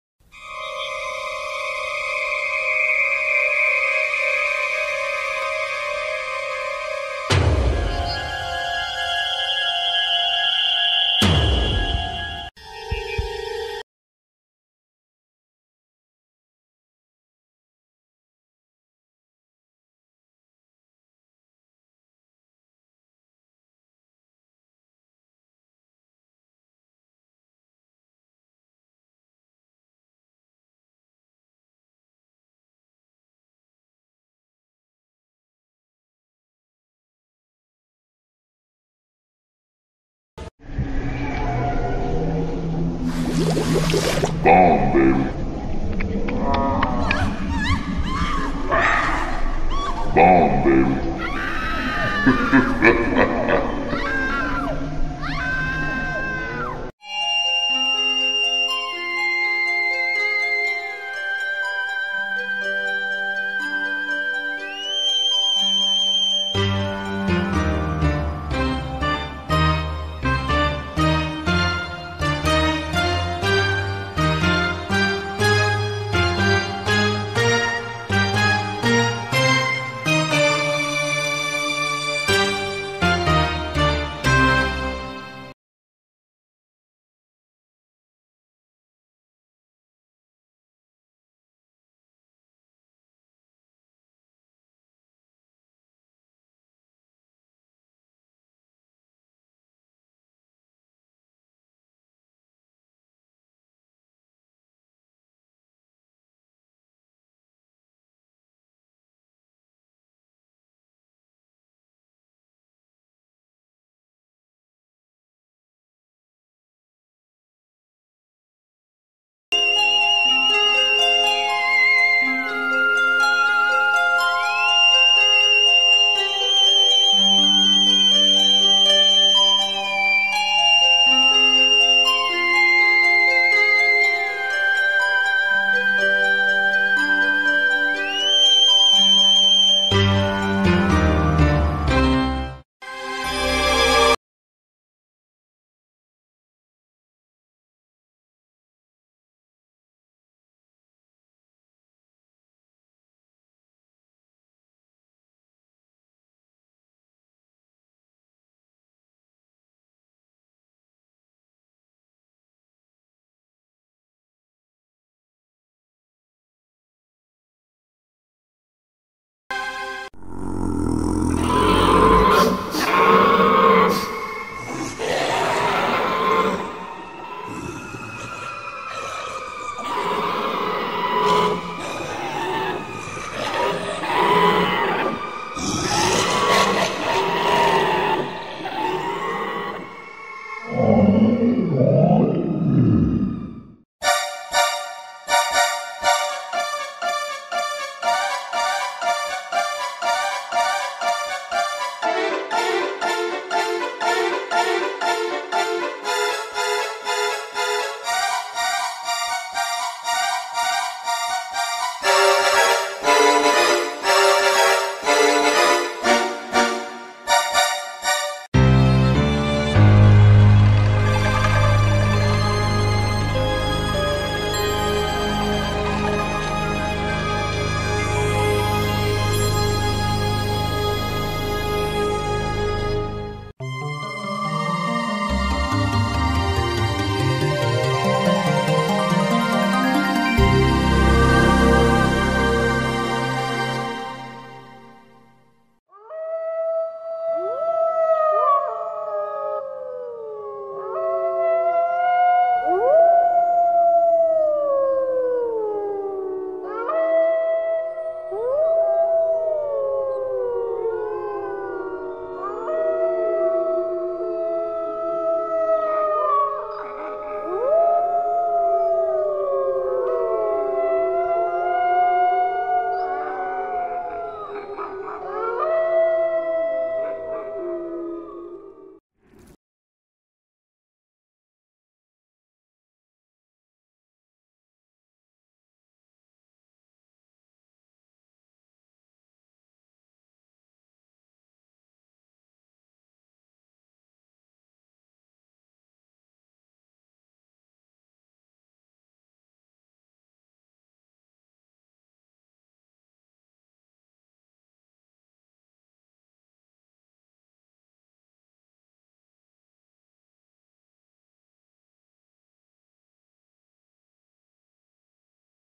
Solo Halloween Night Ride 2013
Look what I found at the streets of Marikina City, Philippines.